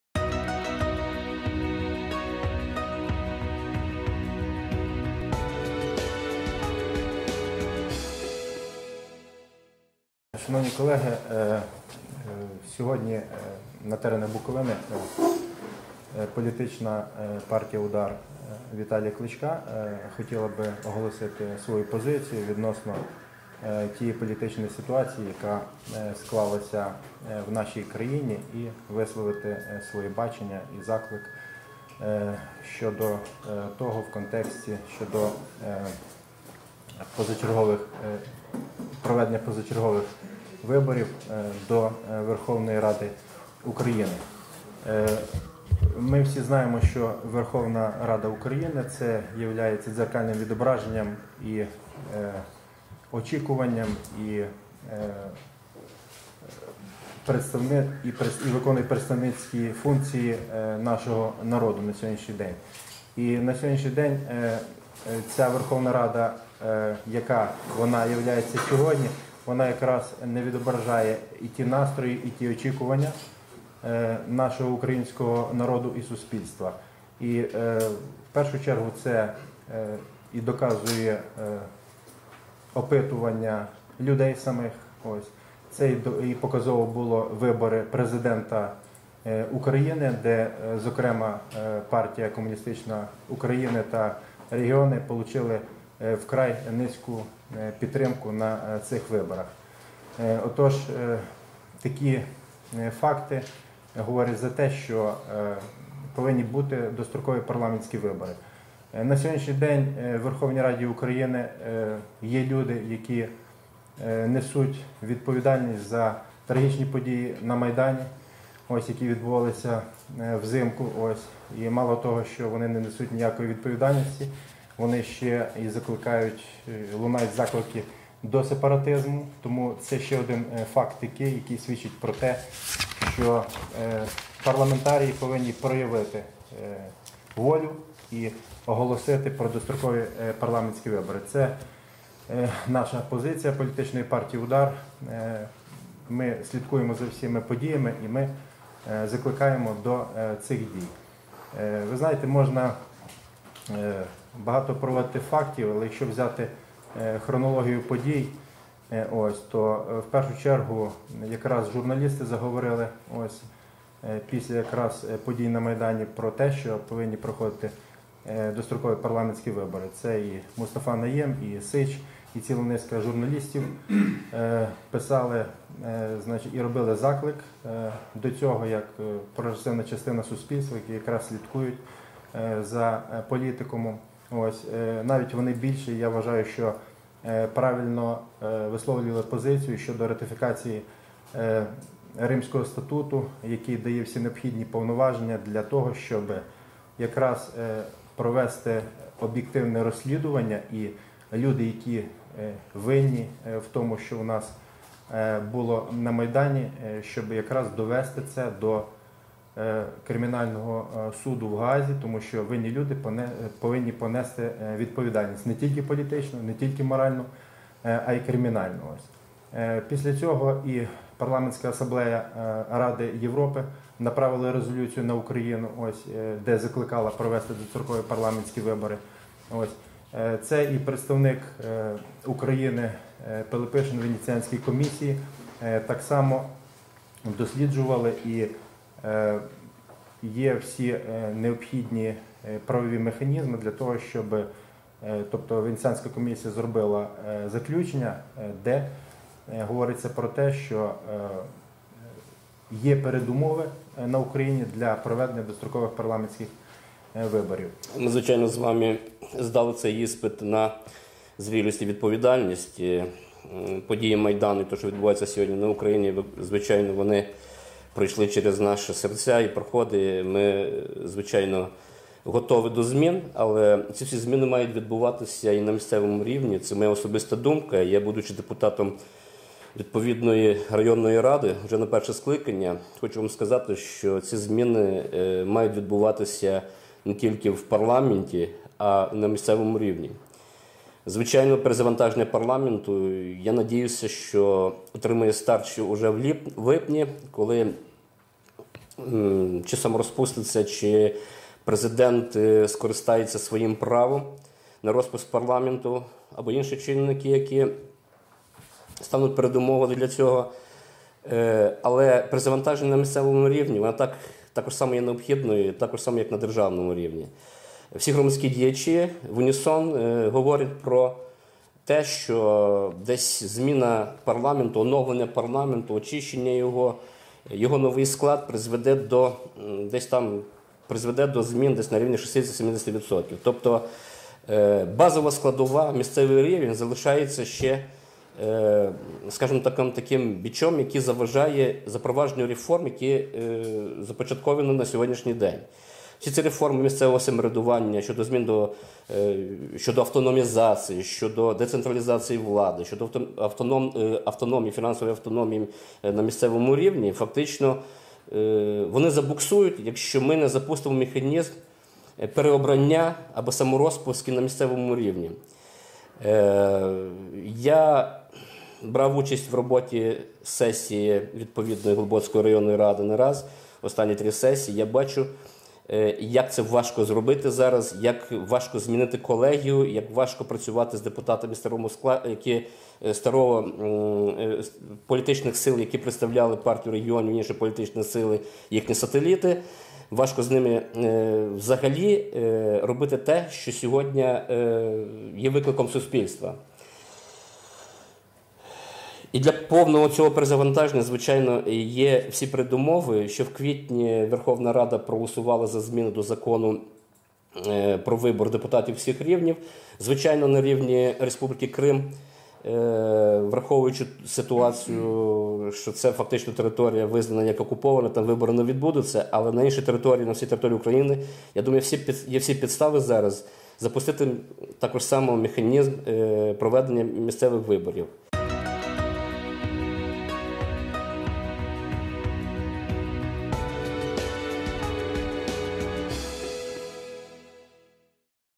У чернівецькому «УДАРІ» переконують, що владу потрібно перезавантажувати
Вчора, 2 липня, у Чернівцях відбувся брифінг заступника голови Чернівецької обласної організації політичної партії «УДАР Віталія Кличка» Володимира Гавреша та голови Глибоцької районної організації Партії, депутата районної ради Григорія Тіміша. Ударівці розповіли своє бачення подальших кроків влади для входження політичного процесу у нормальне русло.